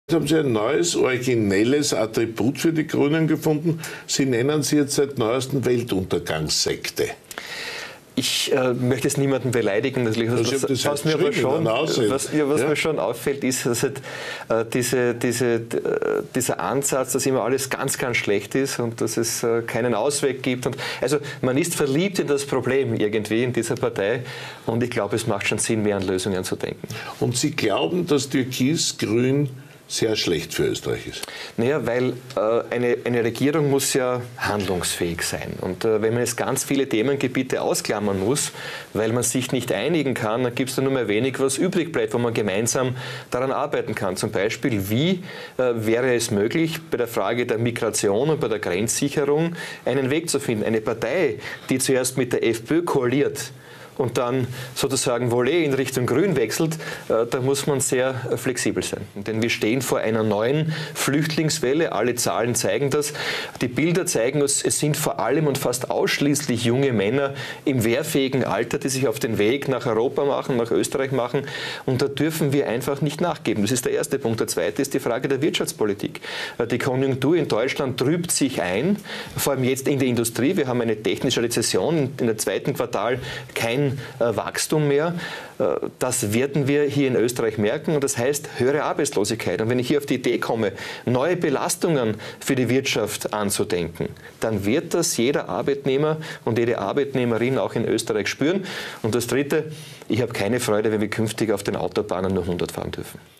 Norbert Hofer: Türkis-Grün ist nicht gut für Österreich!
Wie sich ÖVP und Grüne bei der Flüchtlingspolitik, bei der Migration und beim Grenzschutz einig werden wollen, bleibt spannend. Wie sieht die Zukunft Österreichs aus, wenn die Grünen mitregieren? Erleben wir schon bald eine unverantwortliche Willkommenspolitik und dürfen wir nur noch maximal 100 km/h auf den Autobahnen fahren?